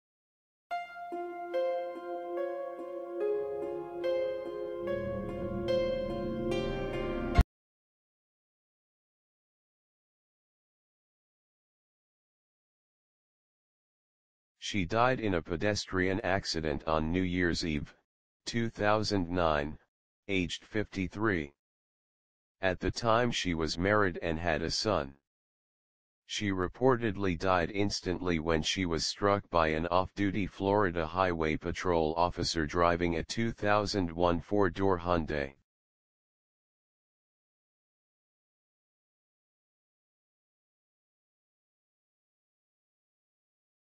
THE DEATH OF ERICA BOYER
This video details the death of porn star Erica Boyer!